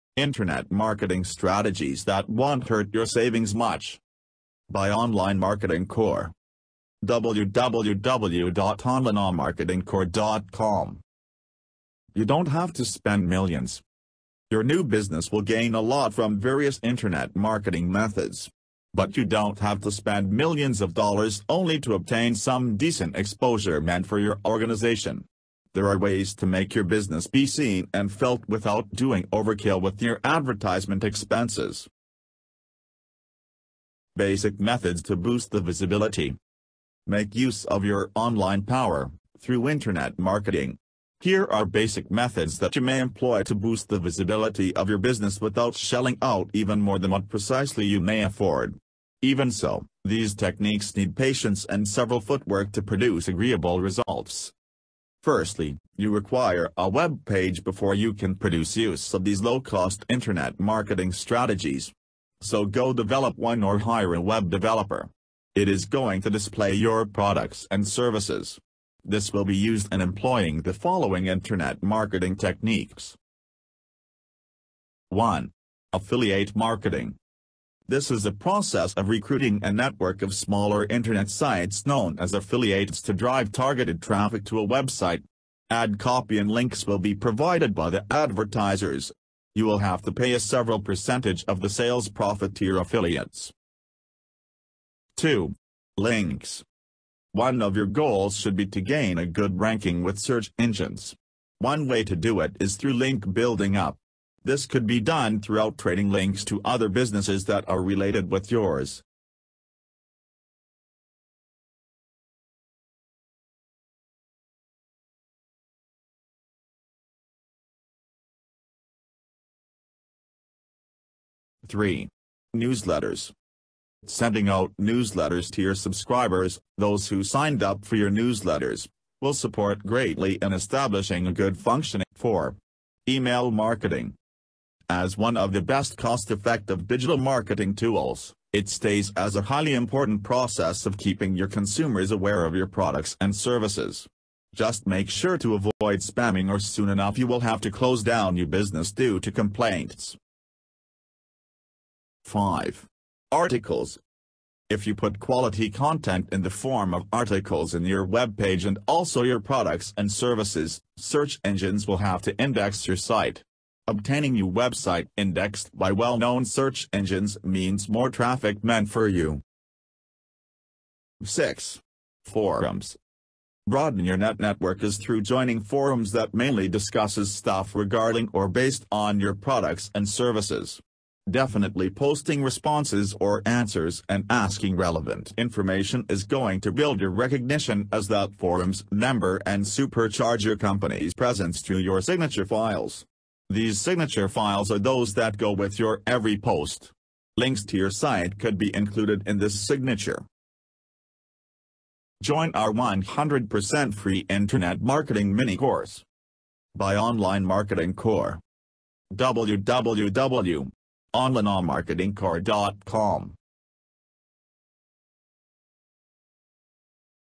Internet Marketing: Internet Marketing Strategies that Wont Hurt Your Savings Much
Your Best Source for Internet Marketing Education.

You don't have to spend millions.

Your new business will gain a lot from various Internet marketing methods. But you don't have to spend millions of dollars only to obtain some decent exposure meant for your organization. There are ways to make your business be seen and felt with out doing overkill with your advertisement expenses.

Basic methods to boost the visibility.

Make use of your online power - through Internet marketing. Here are basic methods that you may employ to boost the visibility of your business without shelling out even more than what precisely you may afford. Even so, these techniques need patience and several footwork to produce agreeable results.

Firstly, you require a web page before you can produce use of these low cost Internet marketing strategies. So go develop one or hire a web developer. It is going to display your products and services. This will be used in employing the following Internet marketing techniques.

2. Links.

One of your goals should be to gain a good ranking with search engines. One way to do it is through link building up. This could be done throughout trading links to other businesses that are related with yours.

3. Newsletters.

Sending out newsletters to your subscribers (those who signed up for your newsletters) will support greatly in establishing a good functioning relationship with your customers or prospective customers. It has to be short, sweet, consistent and written for a general target audience.

4. E-mail Marketing.

As one of the best cost-effective Digital marketing tools, it stays as a highly important process of keeping your consumers aware of your products and services. Just make sure to avoid spamming or soon enough you will have to close down you business due to complaints.

5. Articles.

If you put quality content in the form of articles in your webpage and also your products and services, search engines will have to index your site. Obtaining you website indexed by well-known search engines means more traffic meant for you.

6. Forums.

Broaden your Net network is through joining forums that mainly discusses stuff regarding or based on your products and services. Definitely posting responses or answers and asking relevant information is going to build your recognition as that forum's member and supercharge your company's presence through your signature files. These signature files are those that go with your every post. Links to your site could be included in this signature.